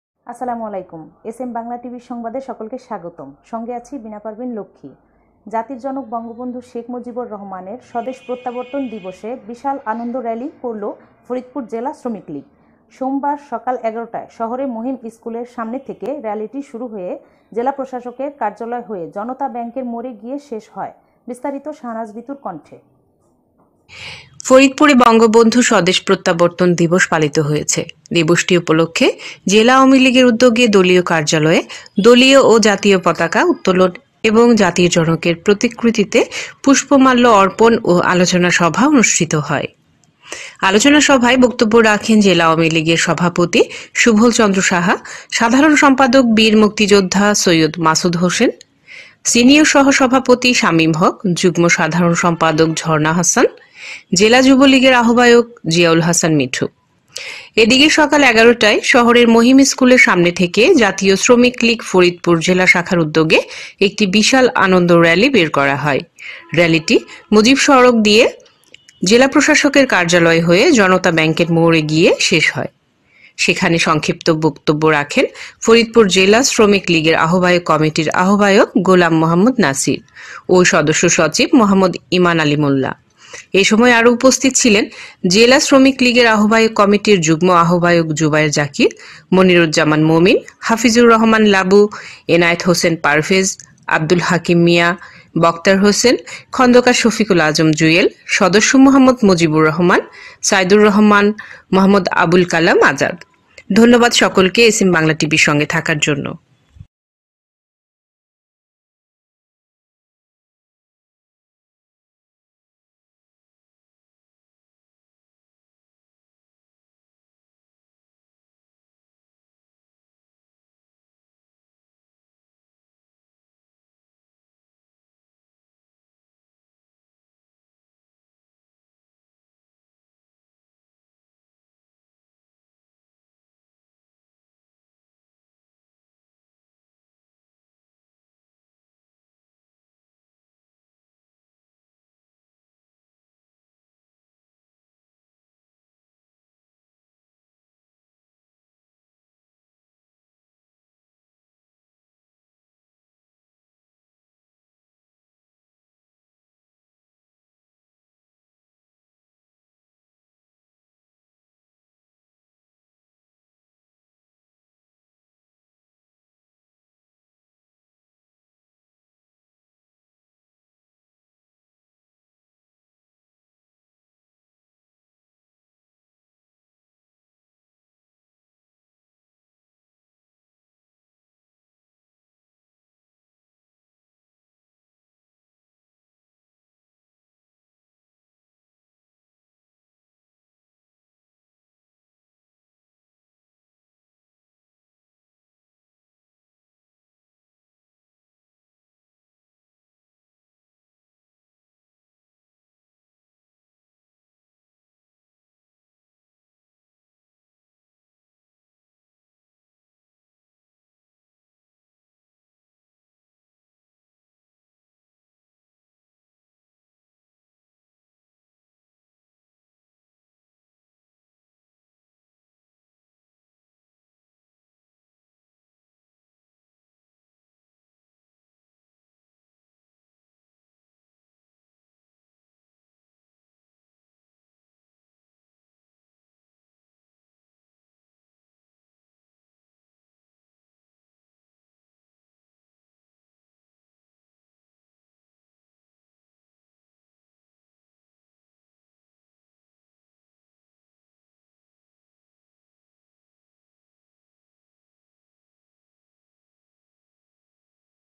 ফরিদপুর বঙ্গবন্ধুর স্বদেশ প্রত্যাবর্তন দিবসে শ্রমিক লীগের আনন্দ র‌্যালী
ফরিদপুর বঙ্গবন্ধুর স্বদেশ প্রত্যাবর্তন দিবসে  শ্রমিক লীগের বিশাল আনন্দ র‌্যালী